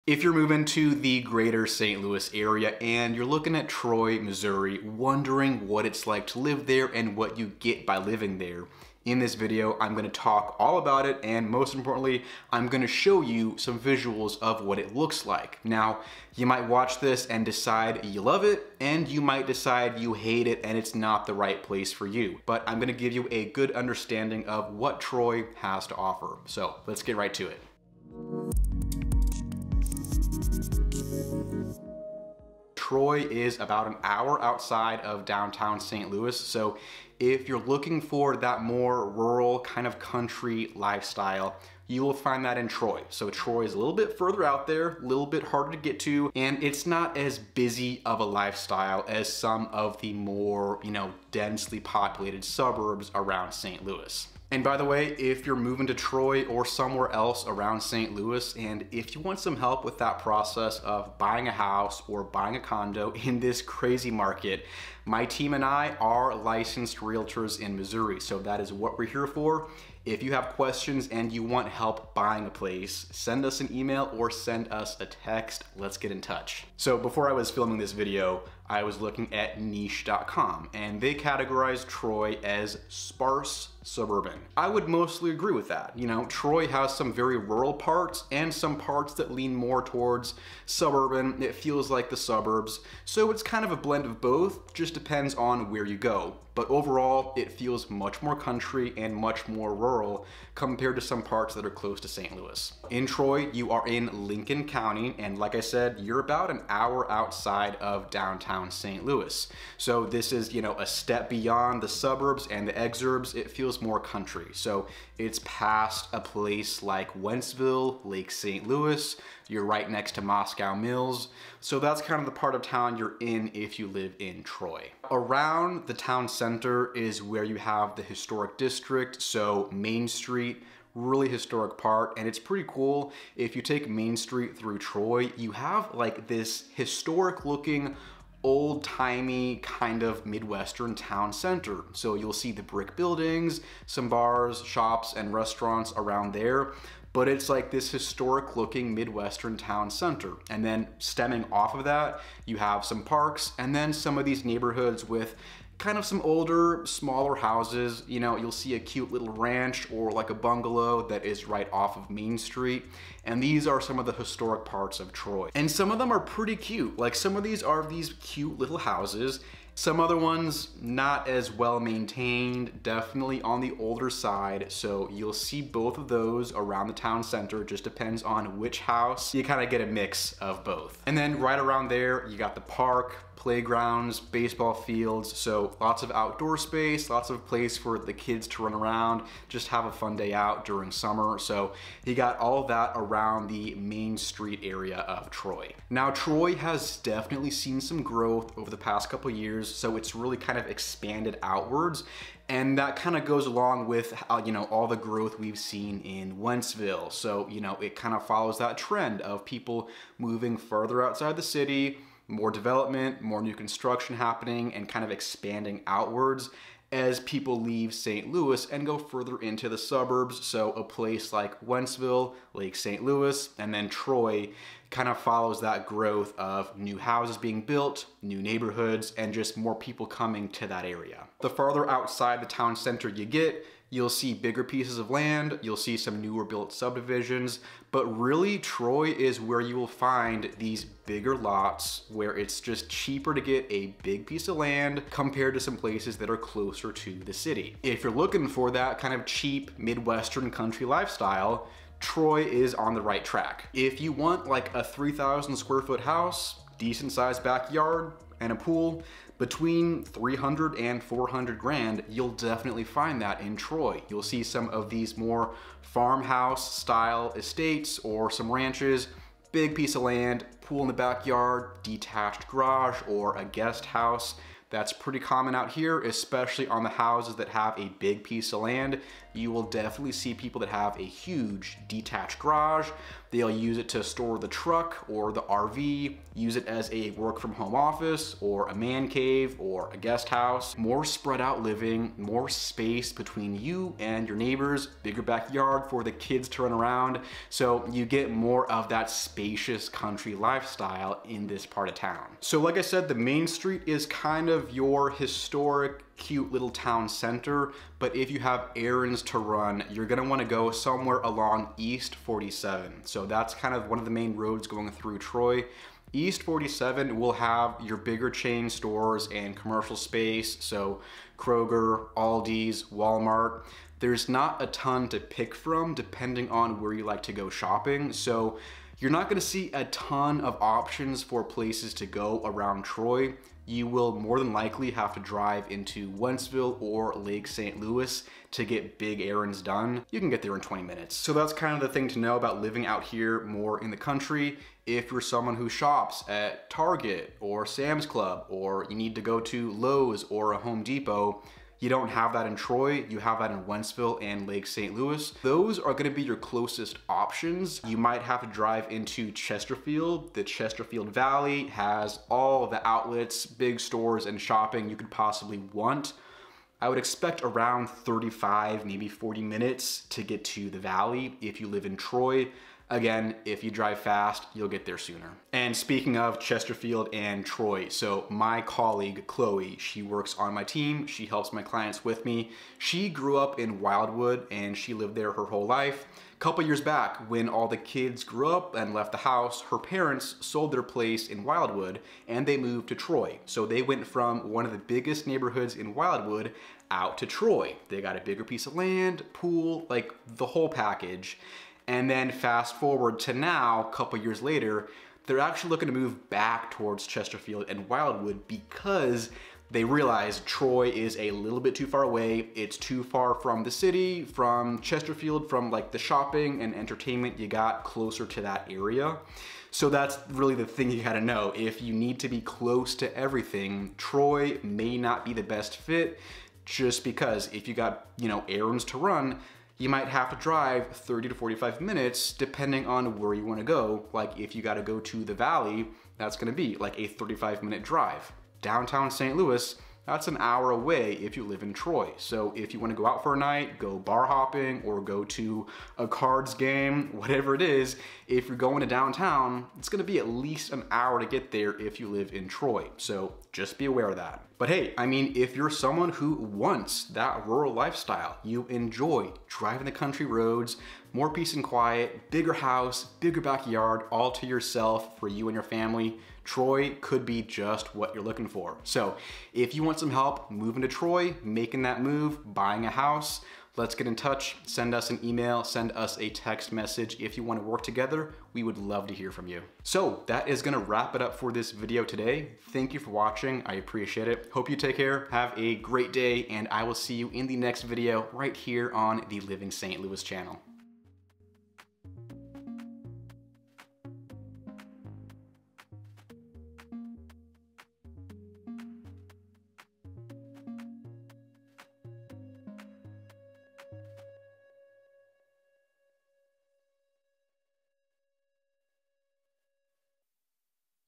What’s it like living in Troy, Missouri? Troy Explained
Today we're talking all about living in Troy, Missouri!  Troy is a beautiful "sparse suburban" town in Lincoln County, Missouri. It's just past Wentzville and Moscow Mills, so it's farther away from Downtown St. Louis roughly an hour outside the city. Troy provides a balance of rural country charm and suburban convenience. It has diverse housing options and unique shops in its historic Main Street district. Expect a calm environment because Troy is not a busy place like some other more densely populated suburbs around St.Louis. Troy, MO offers a more spacious country lifestyle where you can enjoy plenty of backyard space and a lower cost of living than expensive suburbs of St. Louis.

🔶If you have questions or you're moving to St. Louis, get in touch with us🔶

Here’s a breakdown:
0:00 Overview of living in Troy, MO
1:16 Troy is a “sparse suburban” area
3:51 Houses in Troy, MO
5:08 Troy, MO isn’t always the most convenient
7:34 Moving to Troy, MO

📲 If you want to see more content follow me & the team!

Jacob Peistrup is licensed in MO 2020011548, MA 9570920, and IL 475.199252